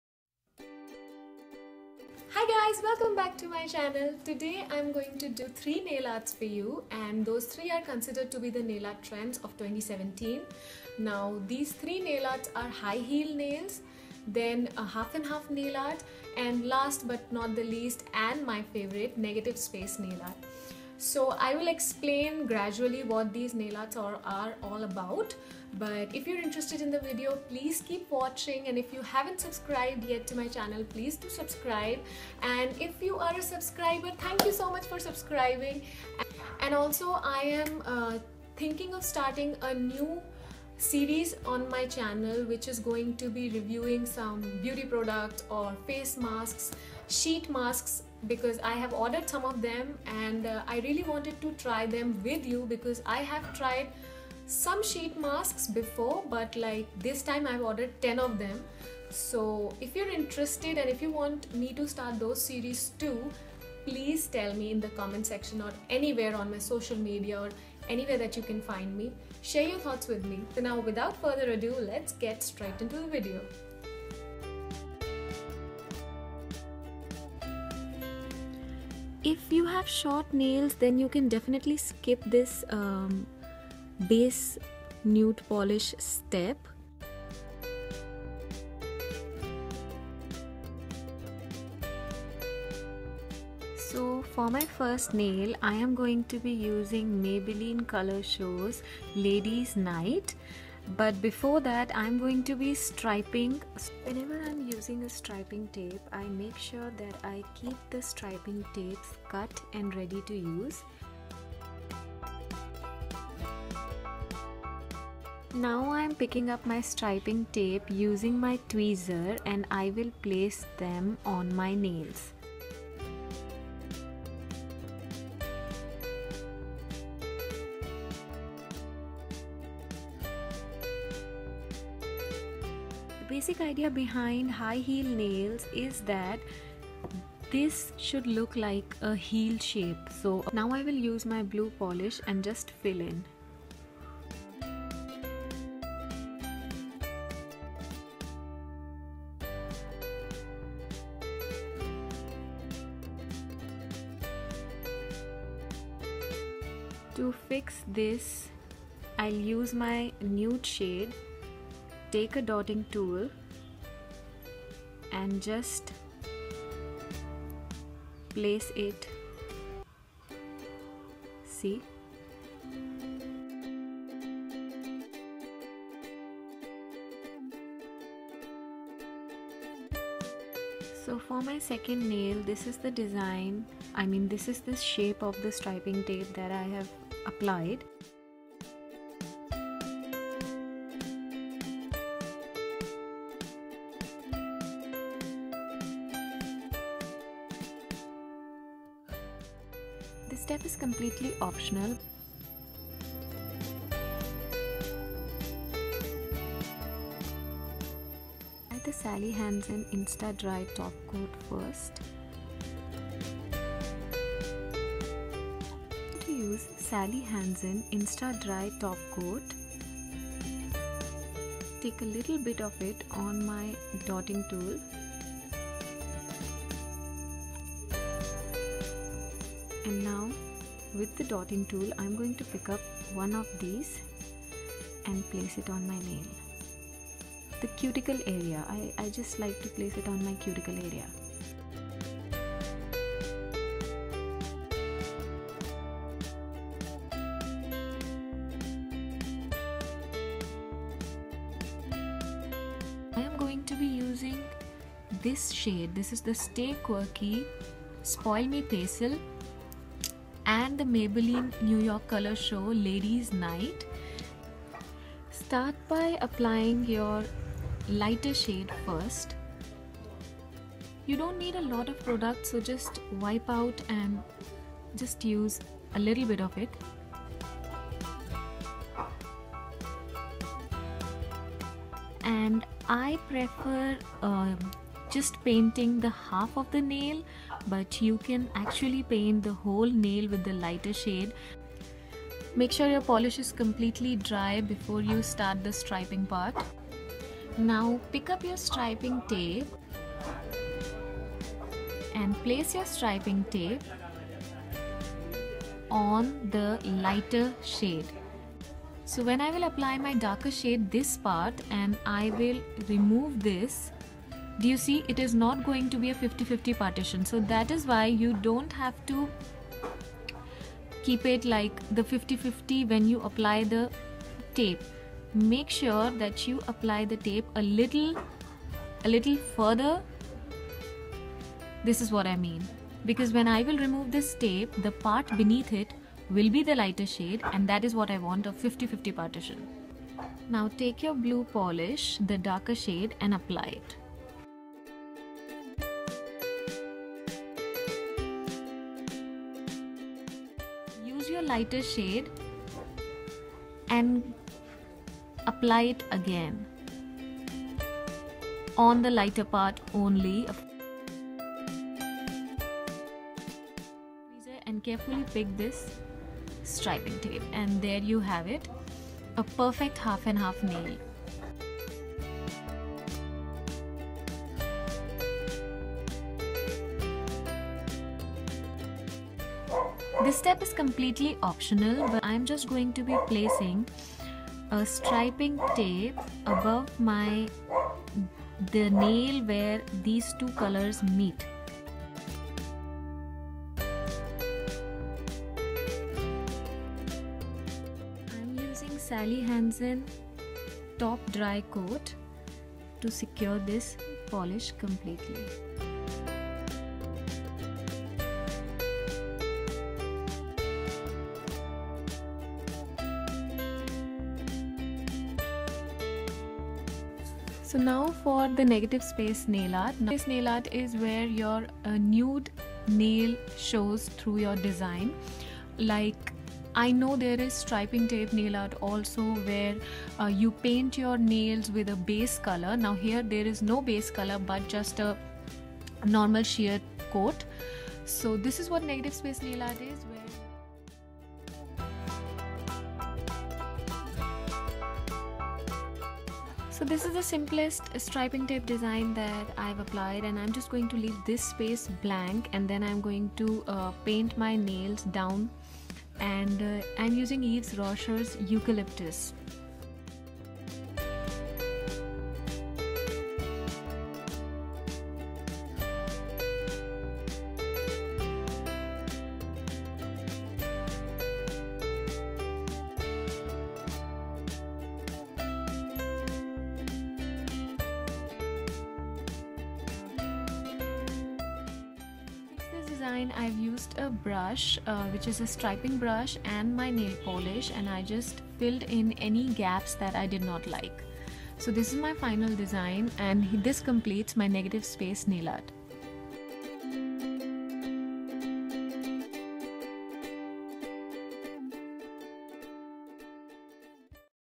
Nail art trends 2017 I High heel nails I Negative space nails I Half n half nails
Hi Nailsit family. In this video I'm going to be help u understand the nail art trends of 2017 (half and half nail art, negative space nail art, high heel nail art). So let's get started, plz share your thoughts and views with me.
love u all xoxo